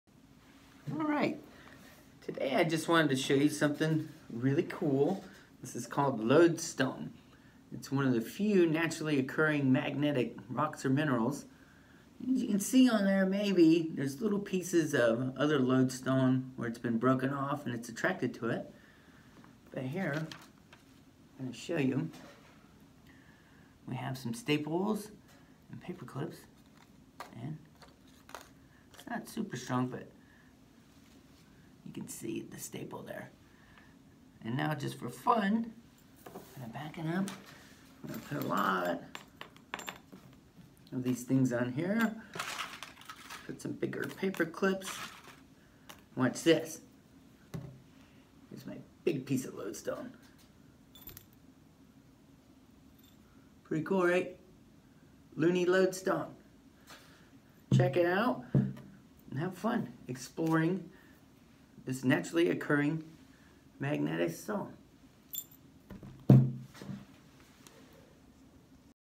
Looney Lodestone Naturally Magnetic Rock Geology Experiment
This quick video will introduce you to lodestone and will demonstrate some of its unique and outrageous properties.